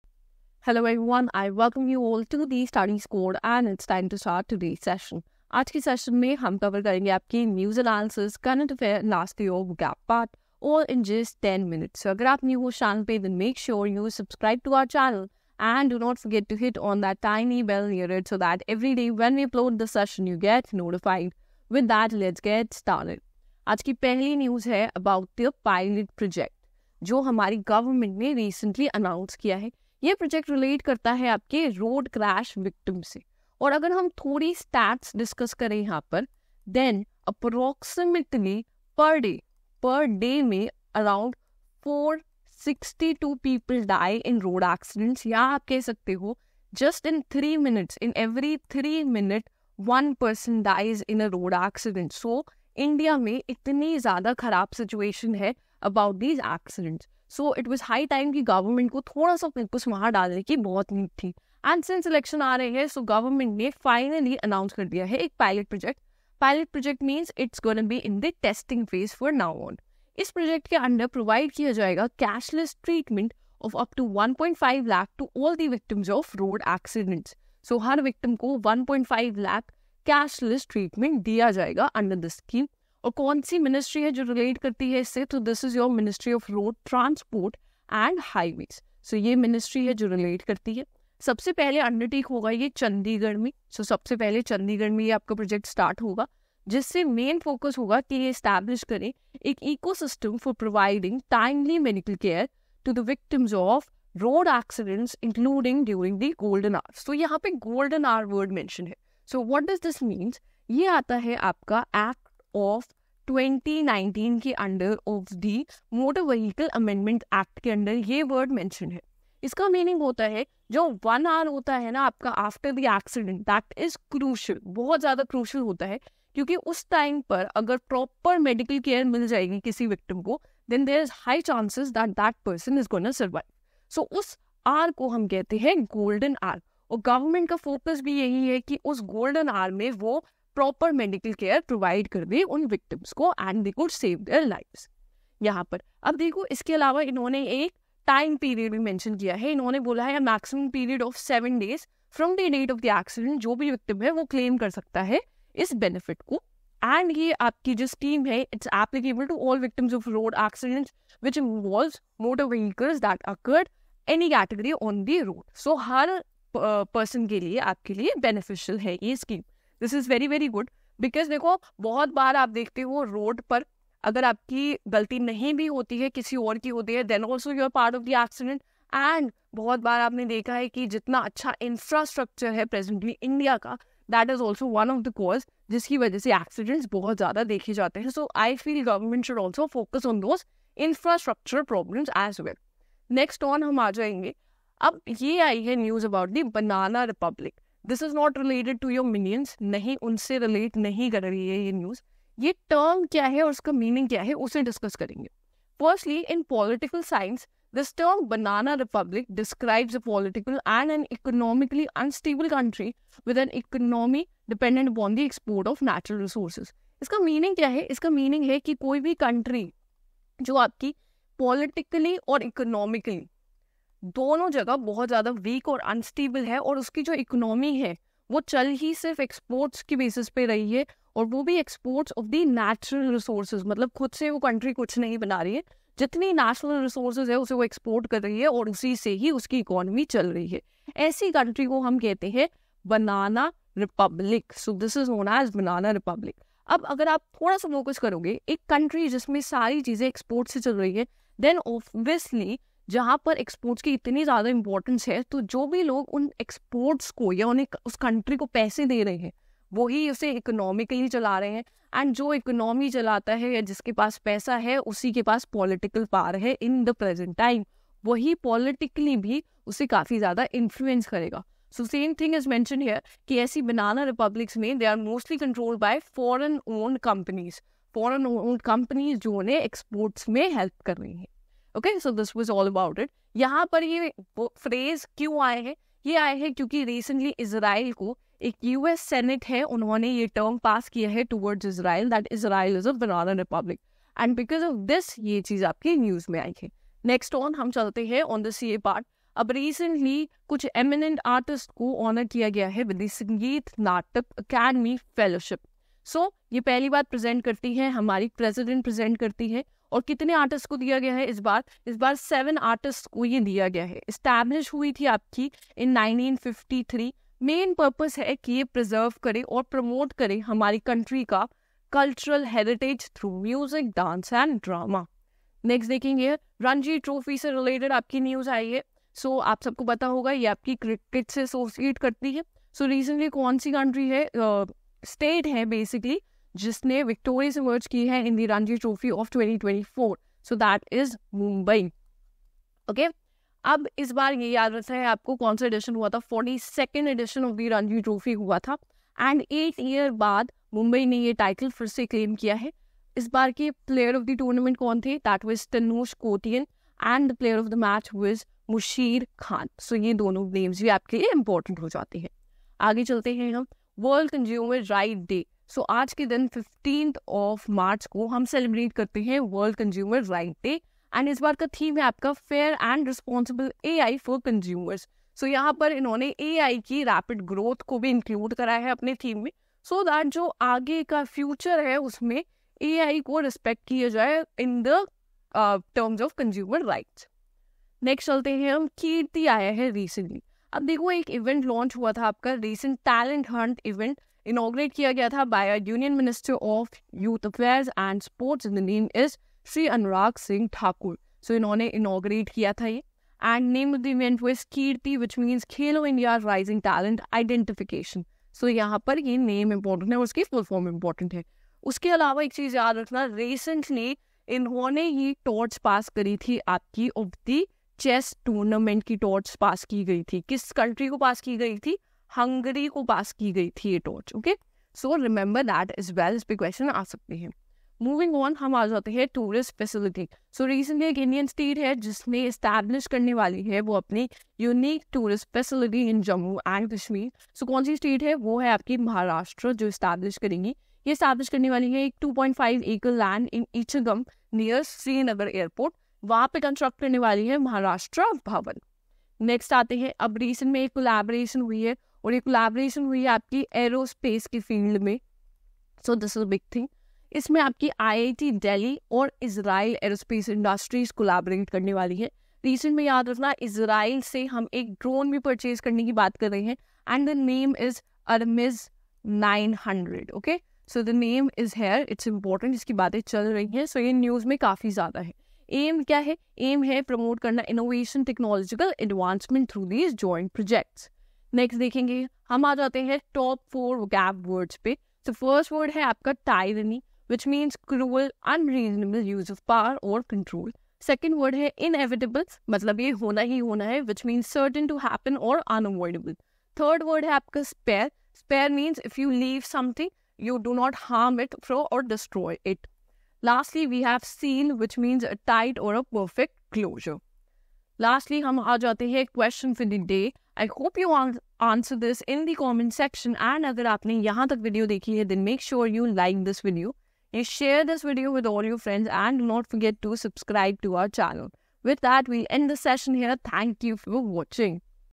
Daily New Analysis + Current Affairs+ Vocab II Under 10 Minutes II For 15th March II 15-3-24 II
Hey there! We've revamped our content to include daily news analysis, current affairs in MCQ format, and a daily dose of vocabulary to simplify your preparation.
We hope you find this new approach helpful. Join our Telegram channel for pdf.